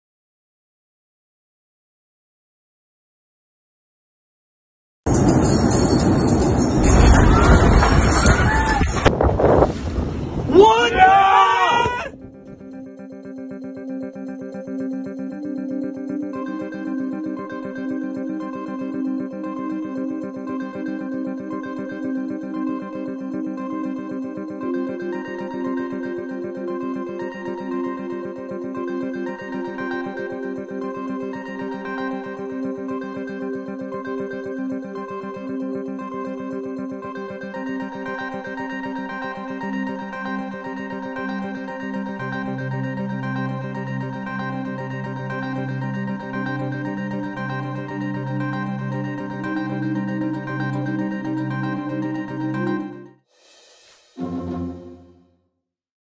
Una gran explosión sacudió Beirut: esto es lo que sabemos hasta ahora
Una gran explosión sacudió a Beirut, la capital de El Líbano, hiriendo a personas, rompiendo ventanas en edificios de toda la ciudad. Videos del momento de la explosión muestran una gran columna de humo en la ciudad.